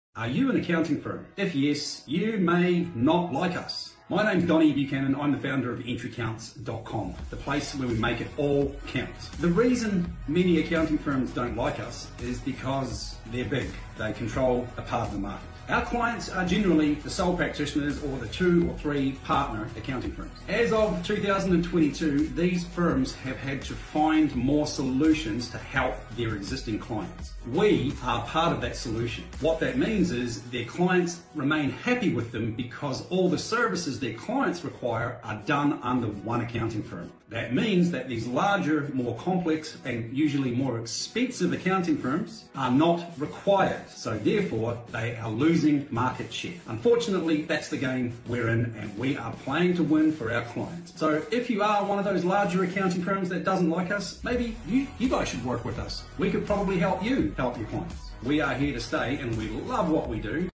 Why some accounting firms don't like us
Accountants,

Our services help small to mid sized accounting firms.

We help them create systems and processes so that they can serve a wider range of clients, most importantly, for audited AASB GPFR.

This enables them to keep all services under one roof, without the competition on their heels.

Plus, we really don't like bureaucracies.